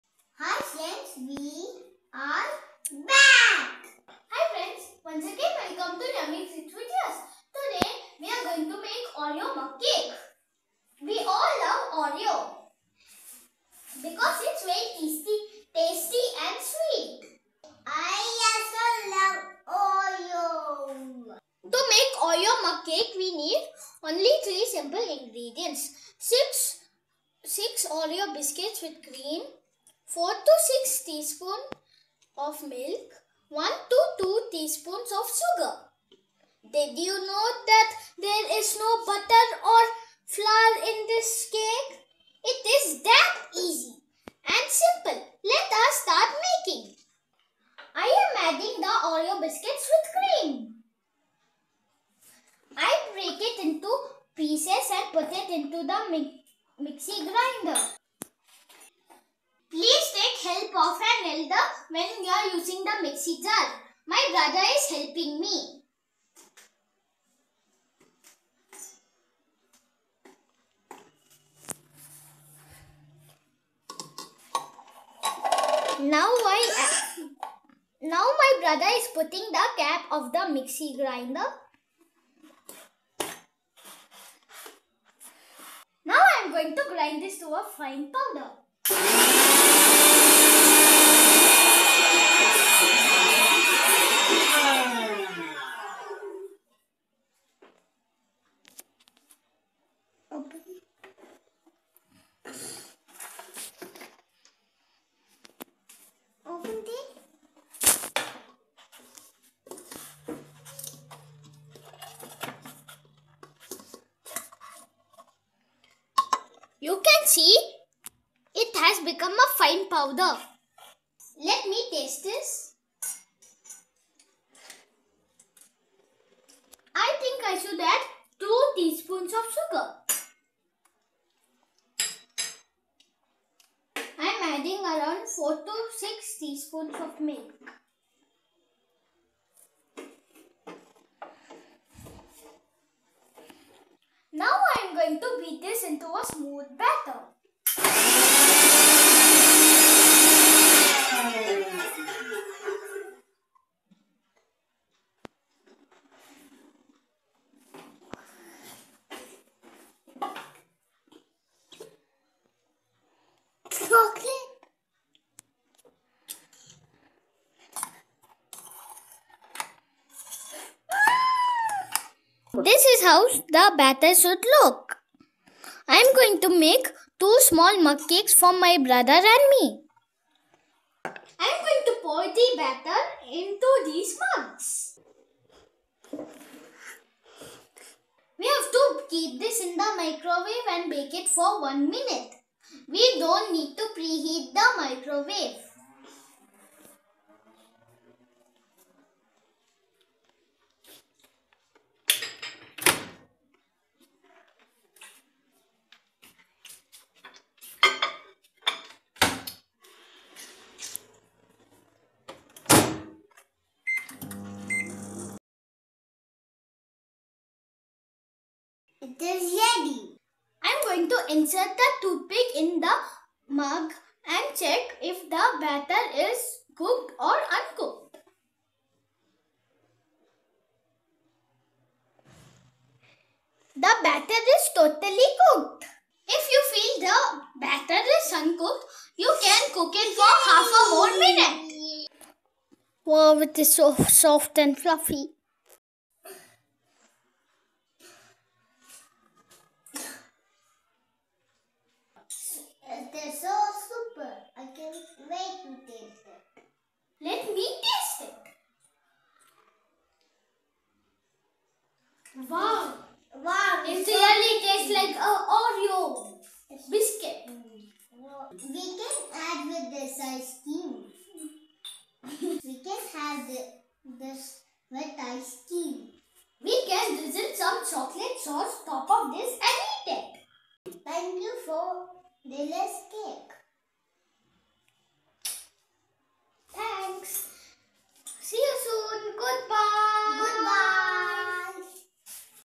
Oreo Mug Cake by Yash
A delightful, mix and stir dessert . Enjoy it with some drizzled chocolate sauce or with a big scoop of vanilla ice-cream ...Yum..Yumm!!